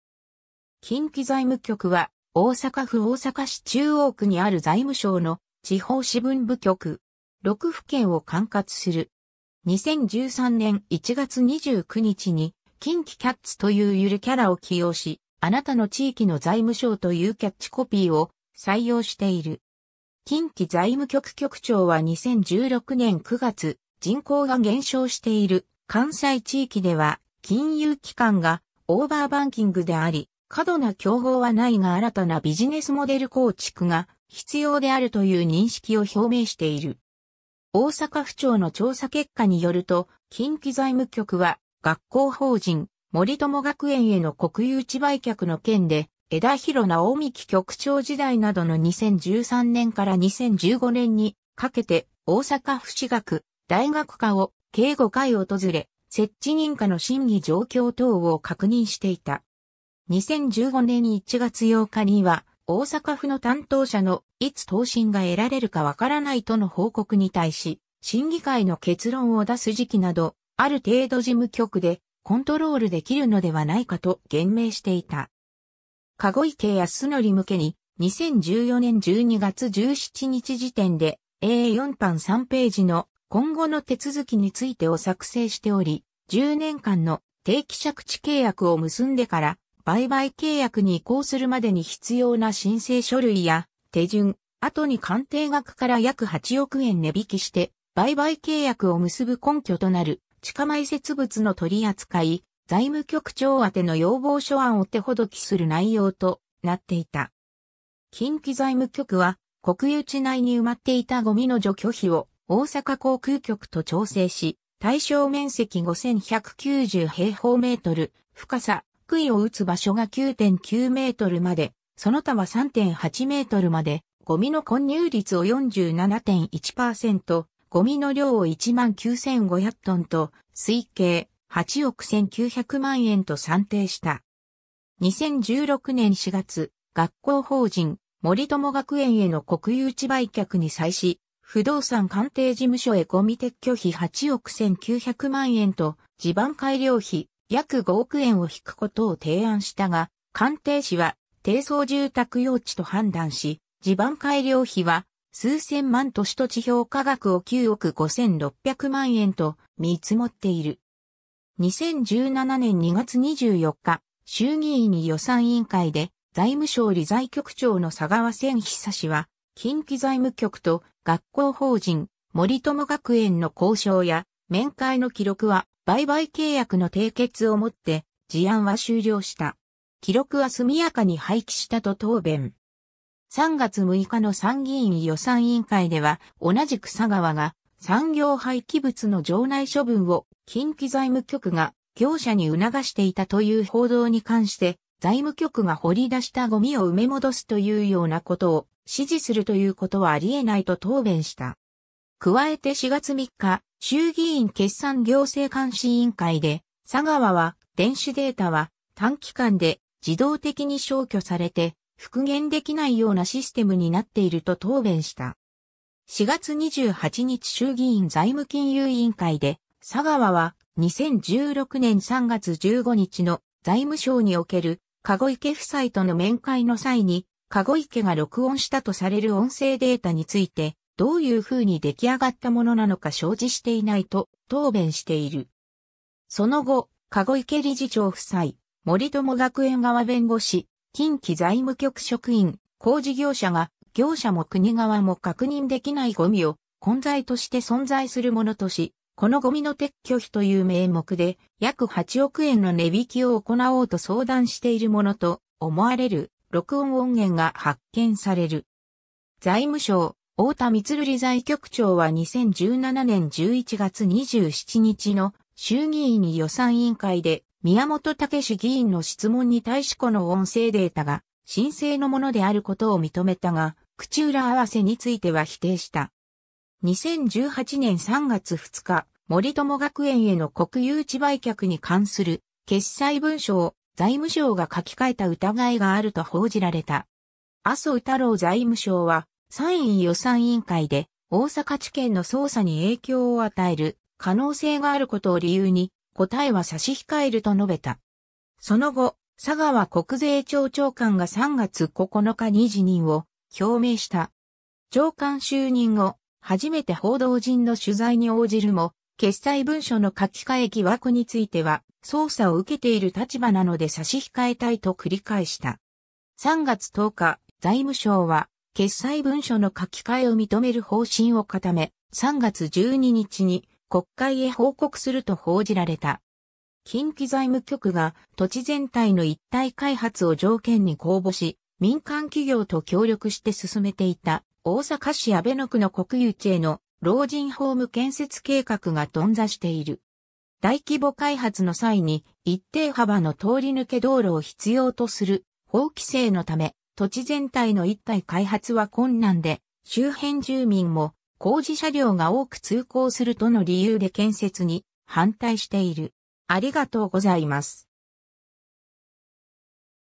近畿財務局
近畿財務局（きんきざいむきょく, Kinki Finance Bureau）は大阪府大阪市中央区にある財務省の地方支分部局。
6府県を管轄する。
 2013年1月29日に「Kinki CATs（キンキキャッツ）」というゆるキャラを起用し、「あなたの地域の財務省」というキャッチコピーを採用している。
 近畿財務局局長は2016年9月、人口が減少している関西地域では、金融機関が「オーバーバンキング」であり、「過度な競合はない」が新たなビジネスモデル構築が必要であるという認識を表明している。
 大阪府庁の調査結果によると、近畿財務局は学校法人森友学園への国有地売却の件で、枝廣直幹局長時代などの2013年から2015年にかけて、大阪府私学・大学課（現：教育庁私学課）を計5回訪れ、設置認可の審議状況等を確認していた。
2015年1月8日には大阪府の担当者の「いつ答申が得られるかわからない」との報告に対し、「審議会の結論を出す時期など、ある程度事務局でコントロールできるのではないか」と言明していた。
 籠池泰典向けに、2014年12月17日時点でA4版3ページの「今後の手続きについて（説明資料）」を作成しており、「10年間の定期借地契約を結んでから売買契約に移行するまでに必要な申請書類や手順」、「後に鑑定額から約8億円値引きして売買契約を結ぶ根拠となる地下埋設物の取り扱い」、「財務局長あての要望書案」を手解きする内容となっていた。
 近畿財務局は国有地内に埋まっていたごみの除去費を大阪航空局と調整し、対象面積5190m2、深さ:杭を打つ場所が9.9mまで、その他は3.8mまで、ごみの混入率を47.1%、ごみの量を19500tと推計、8億1900万円と算定した。
2016年4月、学校法人森友学園への国有地売却に際し、不動産鑑定事務所へごみ撤去費8億1900万円と地盤改良費約5億円を引くことを提案したが、鑑定士は低層住宅用地と判断し、地盤改良費は数千万とし土地評価額を9億5600万円と見積もっている。
 2017年2月24日、衆議院予算委員会で財務省理財局長の佐川宣寿は近畿財務局と学校法人森友学園の交渉や面会の記録は「売買契約の締結をもって、事案は終了した。記録は速やかに廃棄した」と答弁。
3月6日の参議院予算委員会では、同じく佐川が「産業廃棄物の場内処分を近畿財務局が業者に促していた」という報道に関して、「財務局が掘り出したごみを埋め戻すというようなことを指示するということはあり得ない」と答弁した。
 加えて4月3日、衆議院決算行政監視委員会で佐川は、電子データは「短期間で自動的に消去されて、復元できないようなシステムになっている」と答弁した。
 4月28日衆議院財務金融委員会で佐川は、2016年3月15日の財務省における籠池夫妻との面会の際に籠池が録音したとされる音声データについて、「どういう風にでき上がったものなのか承知していない」と答弁している。
 その後、籠池理事長夫妻、森友学園側弁護士、近畿財務局職員、工事業者が、業者も国側も確認できないゴミを「混在」として存在するものとし、このゴミの撤去費という名目で約8億円の値引きを行おうと相談しているものと思われる録音音源が発見される。
財務省太田充理財局長は2017年11月27日の衆議院予算委員会で宮本岳志議員（日本共産党）の質問に対しこの音声データが真正のものであることを認めたが、「口裏合わせ」については否定した。
 2018年3月2日、森友学園への国有地売却に関する決裁文書を、財務省が書き換えた疑いがあると報じられた。
麻生太郎財務相は、参院予算委員会で大阪地検の捜査に影響を与える可能性があることを理由に「答えは差し控える」と述べた。
その後、佐川国税庁長官が3月9日に辞任を表明した。
長官就任後、初めて報道陣の取材に応じるも、決裁文書の書き換え疑惑については「捜査を受けている立場なので差し控えたい」と繰り返した。
3月10日、財務省は決裁文書の書き換えを認める方針を固め、3月12日に国会へ報告すると報じられた。
 近畿財務局が土地全体の一体開発を条件に公募し、民間企業と協力してすすめていた、大阪市阿倍野区の国有地への老人ホーム建設計画が頓挫している。
大規模開発の際に一定幅の通り抜け道路を必要とする法規制のため、土地全体の一体開発は困難で、周辺住民も「工事車両が多く通行する」との理由で建設に反対している。